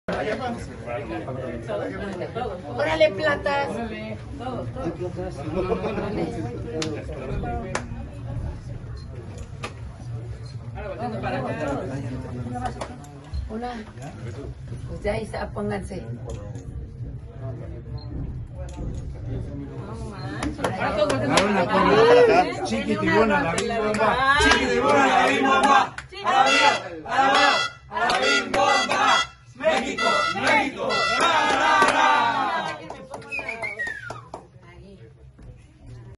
París 2024: México festeja medalla de plata de Juan Celaya y Osmar Olvera
"¡Chiquitibum a la bim bom bam, México, México!"

El equipo mexicano celebró con porras y selfies la medalla conseguida por la dupla mexicana Juan Celaya y Osmar Olvera en trampolín de tres metros.

Imágenes de Reforma / EXPRESO.